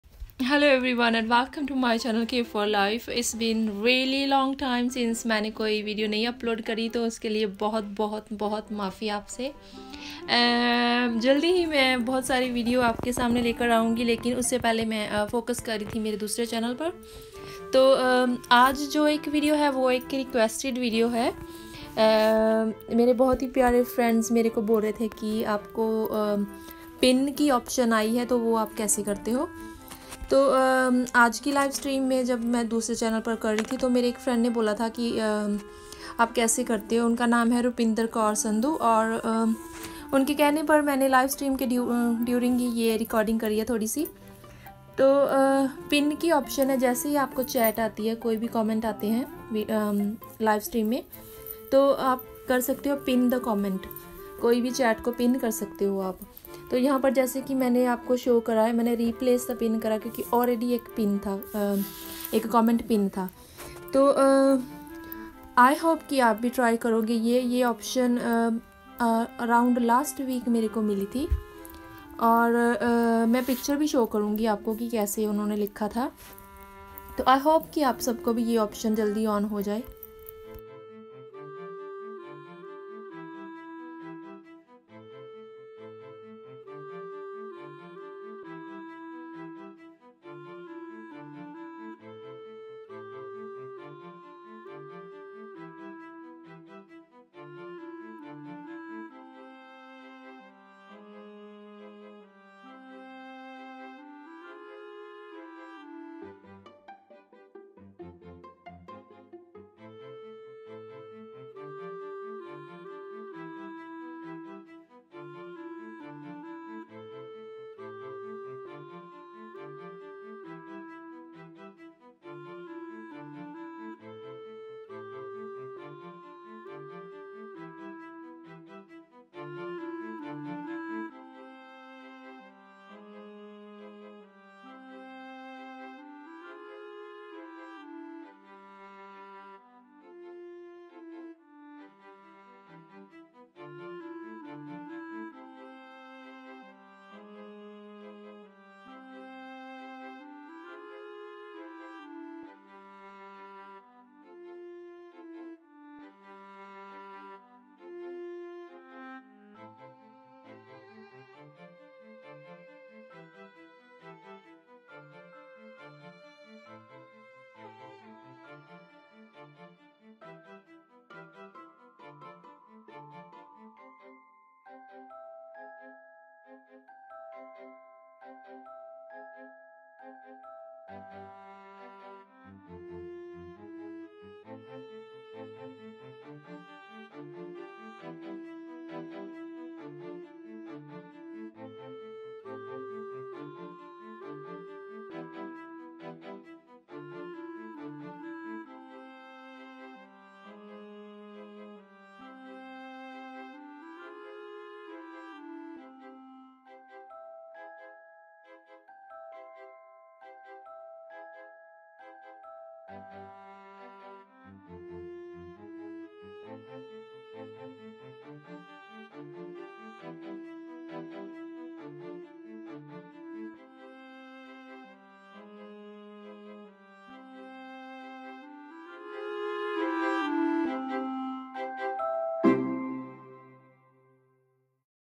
Pin a comment in live chat || new pin option || English Captions 😊
First of all Happy International Women’s Day to all the lovely women out there🤗😍🍫

Today’s video topic:

How I pin a message in Live Chat

You can pin a message for some limited amount of time,replace to another one
Or you could leave the same message the one you want to highlight throughout the live stream

To pin a live chat message
You will click on a channel name in live chat messages
Then right click and click on pin the message option

Hope you’ll find it useful
see you all till my next video comes

This video’s purpose is just for informational use only
During a live stream I was asked to show how I pin live chat messages on our other channel
Therefore I recorded and posted this video for you

Timestamps:
0:00 Quick Introduction
0:19 Explanation on why I am showing this
0:45 How to pin a comment in live
1:02 Thank you for watching
1:20 Footage of Live Stream and Pin examples
3:45 Pin chat alert screenshot

Note for my viewers:

this pin option got enabled on our second channel as channel name shows in the video too
Video’s purpose is for the sake of information/ educational only
It show how it works not how to get the option enabled from Youtube
Hope this answers your queries😊 Be sure to

And set the bell icon 🔔 to all so you never miss an update on my yt channel!